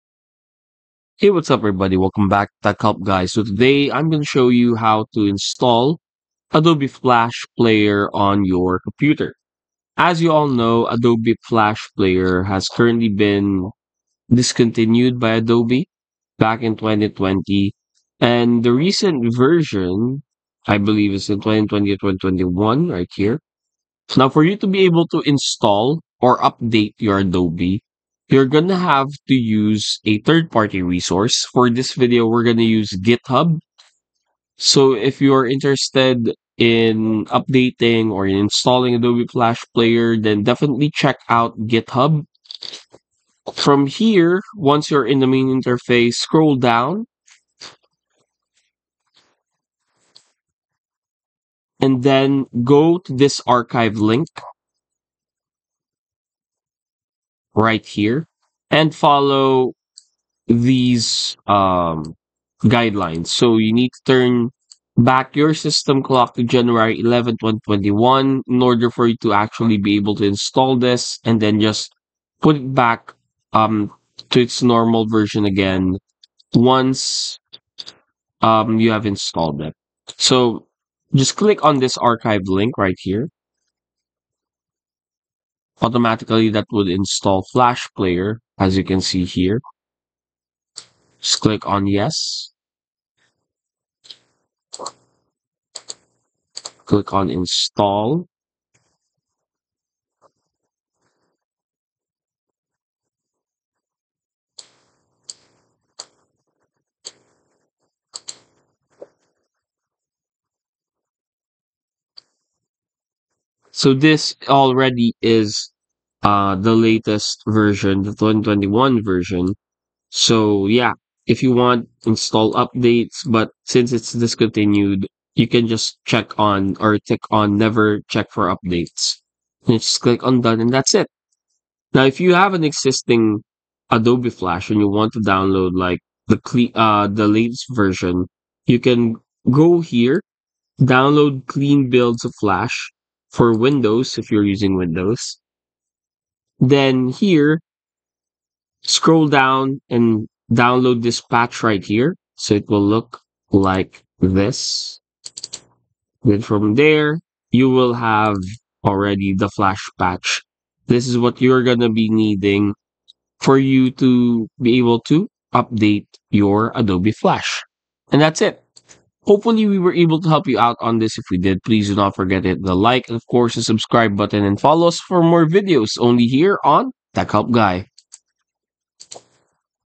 How To UPDATE Adobe Flash Player On Windows 10, 11 (2025)
In this video tutorial, I'll show you how to update Adobe Flash Player on Windows 10 and 11. The method is very simple and clearly described in the video. How To UPDATE Adobe Flash Player On Windows 10, 11 (2025). Regardless of your device, you will learn how to update Flash Player for better performance, and how to ensure your browser is compatible with the update.